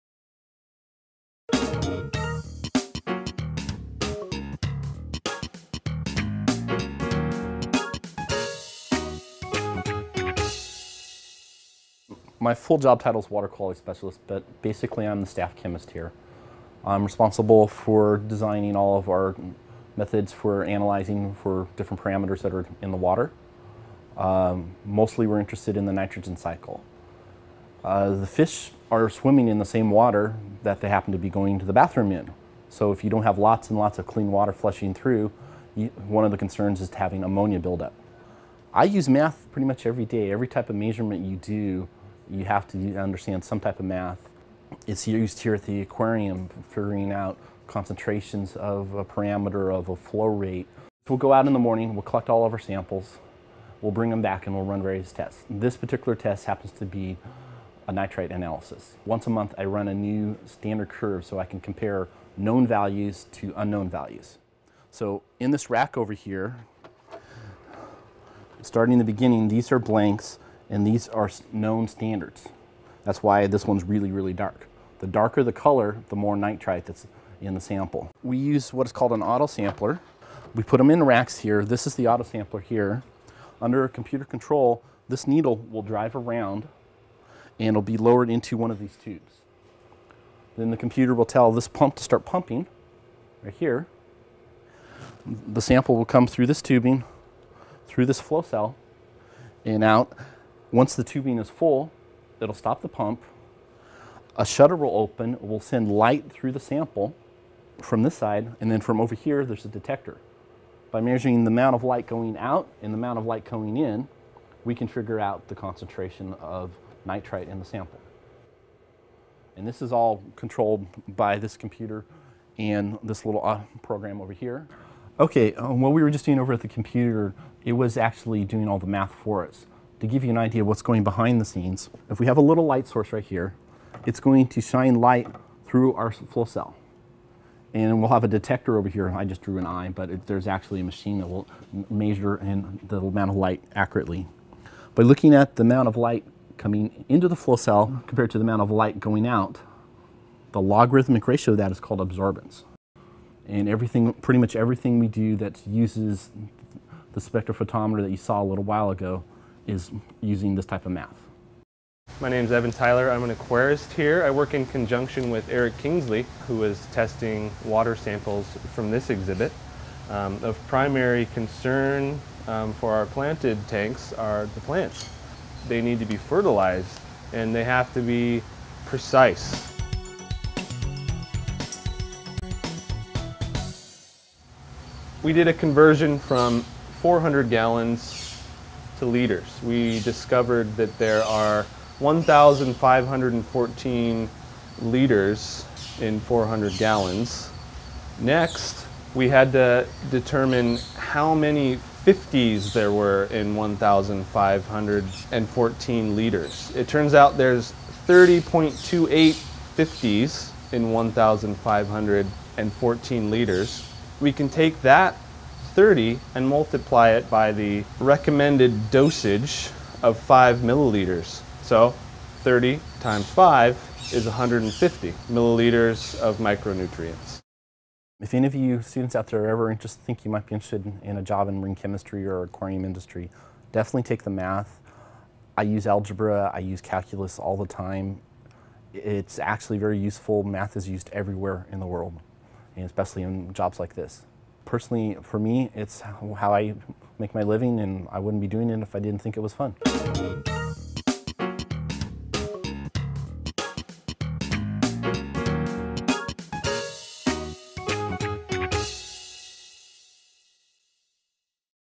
'Math in the real world ' Monterey Bay Aquarium & MCOE
The Monterey Bay Aquarium and The Monterey County Office of Education present 'Math in the Real World'. Evan Tyler and Eric Kingsley of the Monterey Bay Aquarium discuss how basic math and conversions are often used in a workplace setting.  Used as a Classroom Supplement for Elementary School Teachers.